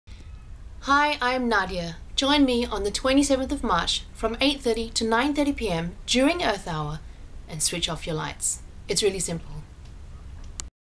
Nadya Hutagalung Earth Hour 2010
Nadya Hutagalung promoting Earth Hour 2010, Singapore.